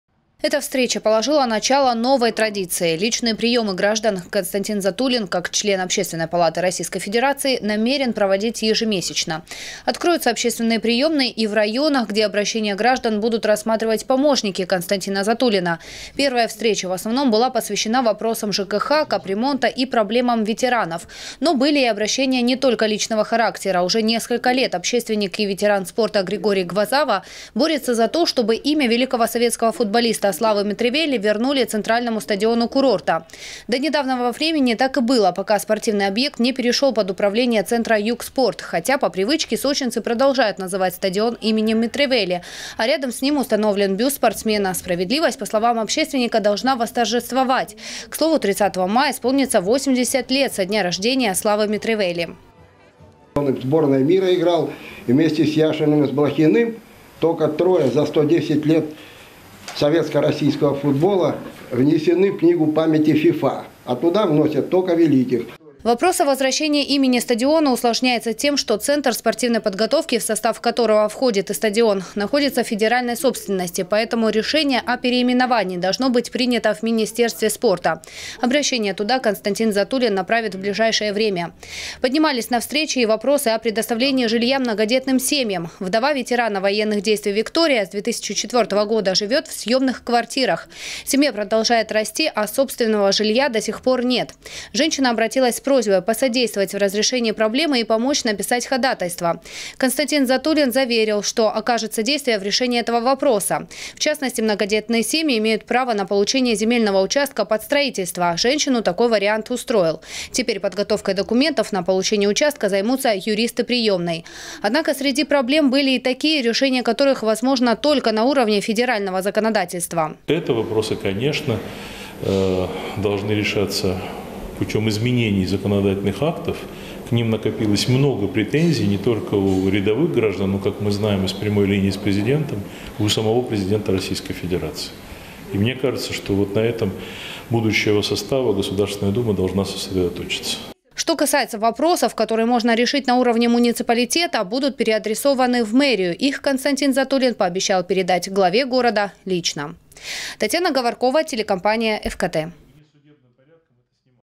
С сочинцами встретился член общественной палаты Константин Затулин. Новости Сочи Эфкате
Общественная приёмная «Единой России» работает в Сочи с прошлого года. Сюда сочинцы обращаются с вопросами как крупного так и более мелкого значения.
Сочи. Новости.
Телеканал Эфкате.